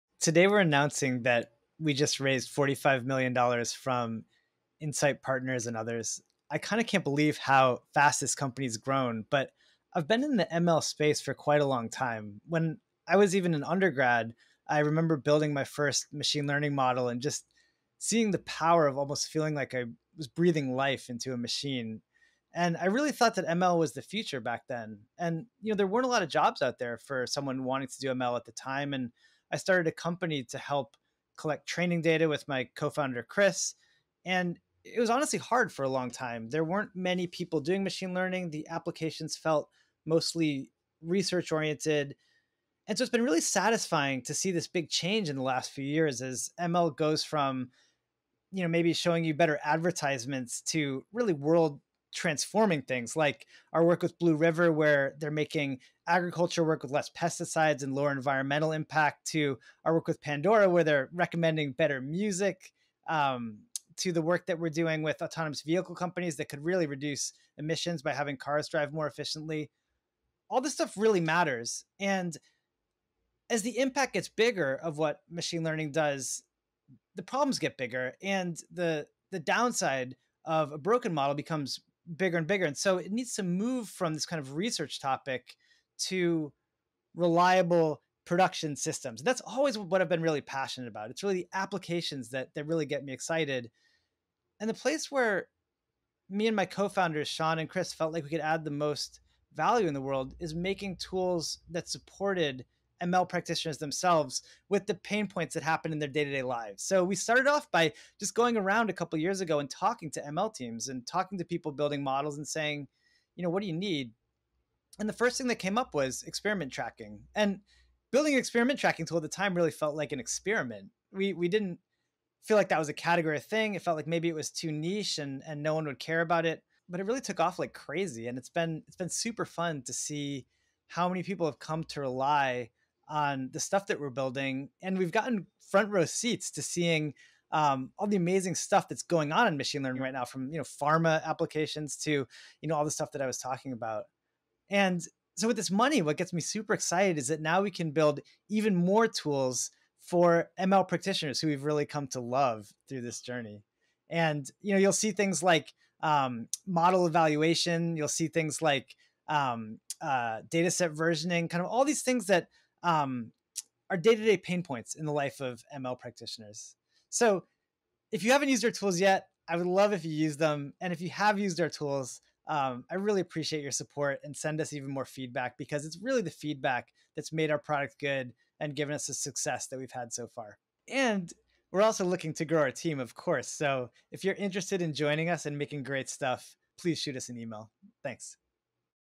W&B raises $45 million to build better tools for ML engineers
If you’re interested in joining us, trying our tools, or have any feedback on our tools, we would love to talk to you. A couple places you can get started: